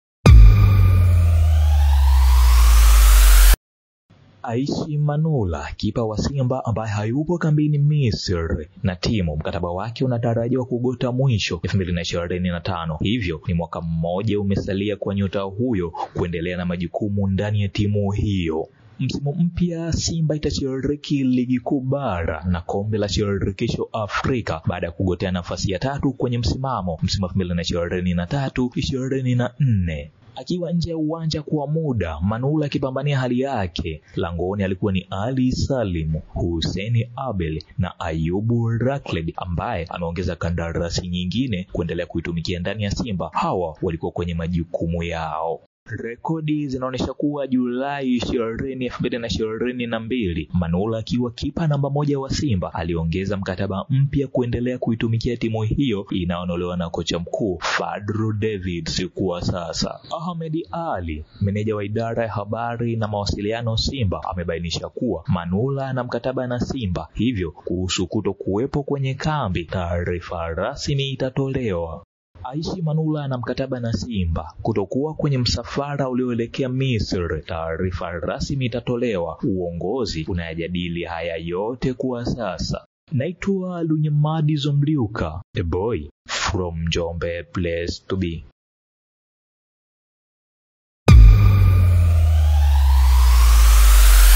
AISHI MANULA SAKATA LAKE SIMBA LIPO HIVI/ MKATABA UPO
AISHI Manula sakata lake Simba lipo namna hii ambapo mkataba wake bado upo lakini ameachwa Bongo.